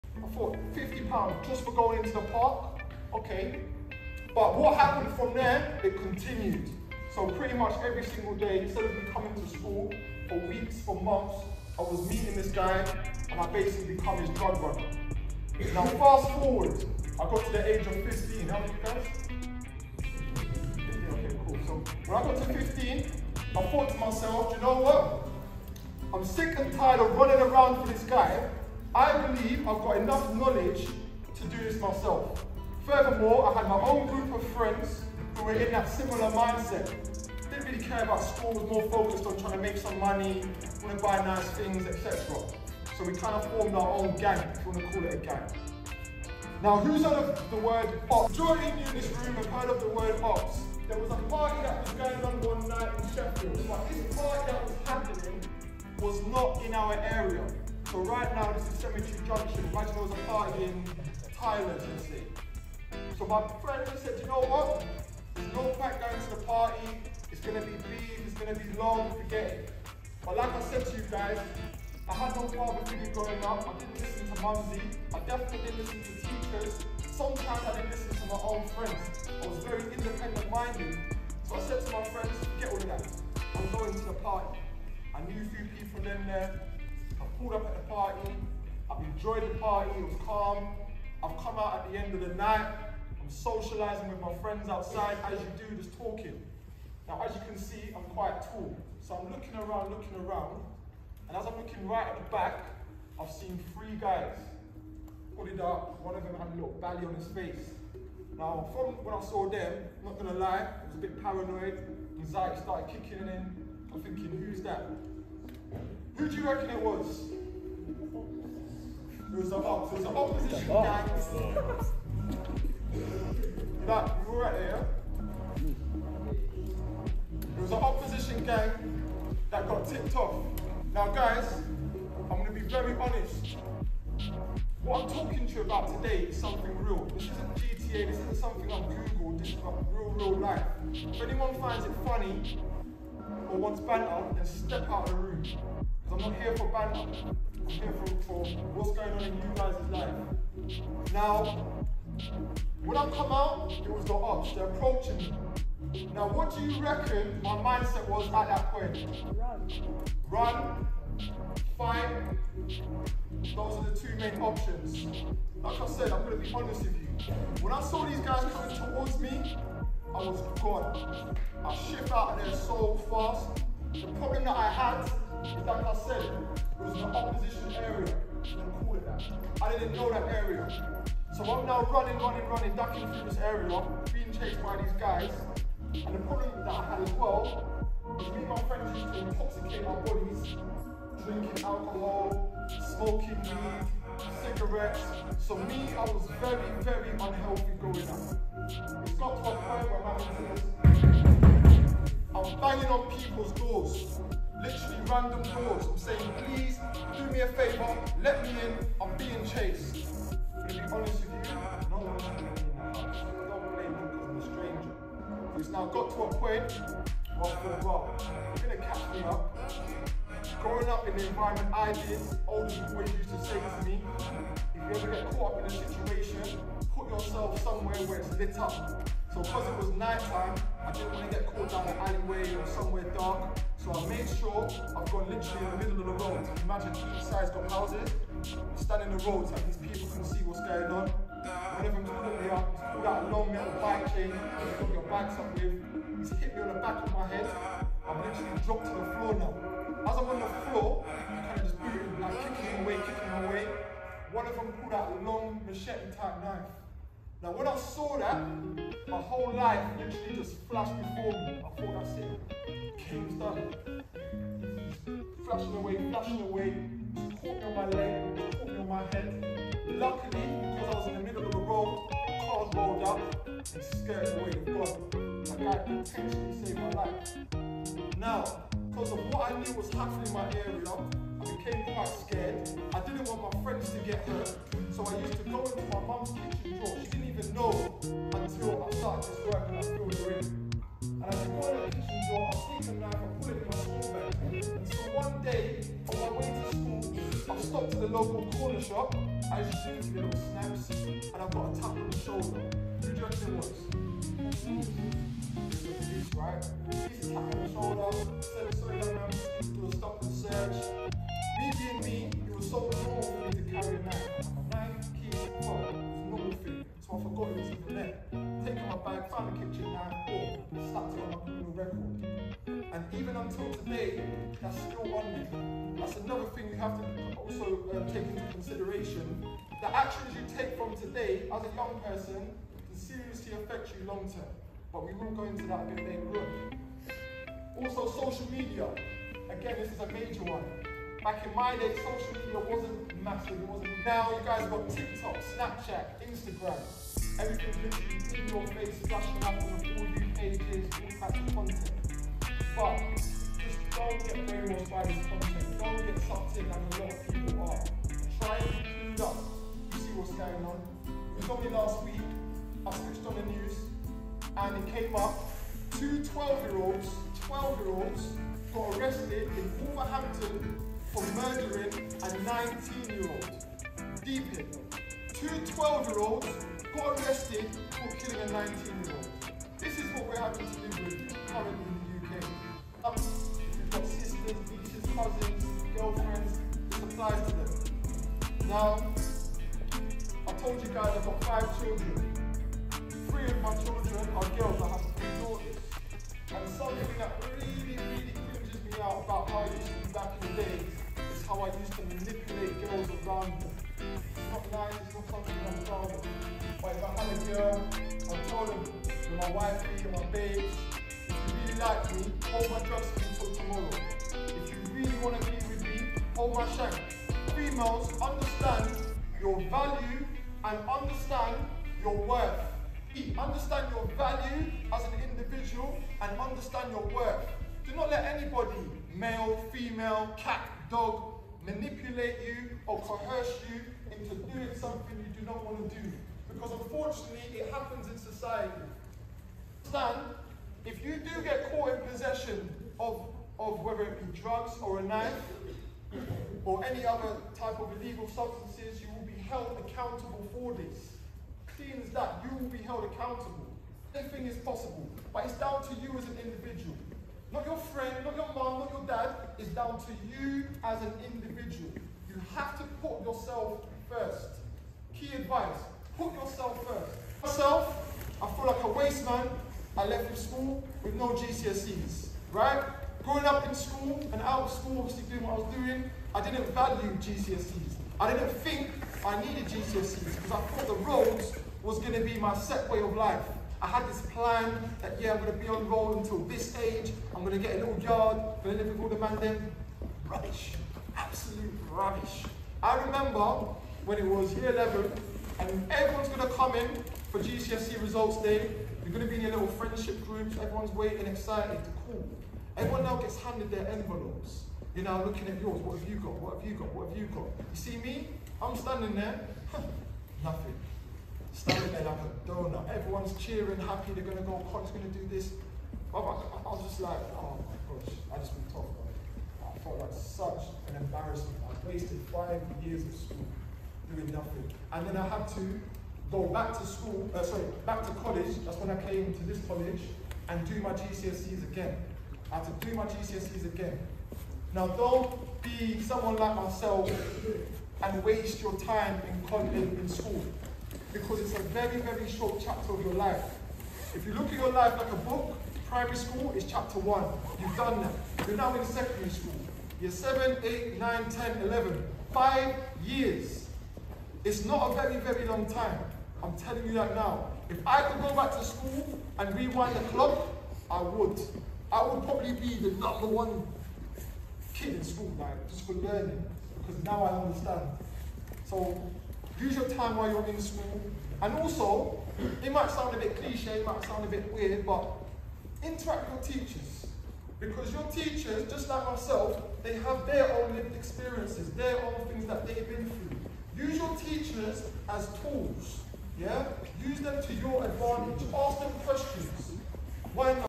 WEAPONS IN SCHOOL | KNIFE CRIME | LIVED EXPERIENCE
Short insight into recent work we have been doing working alongside Storyy Group.

Entering Secondary Schools And Colleges nationwide, we have been discussing social pressures and solutions for our young people nationwide from a lived experienced angle.

Topics include :

- Gang culture
- Weapon carrying
- Realities of the justice system
- County Lines (Drug running)
- Bullying
- What are my options ?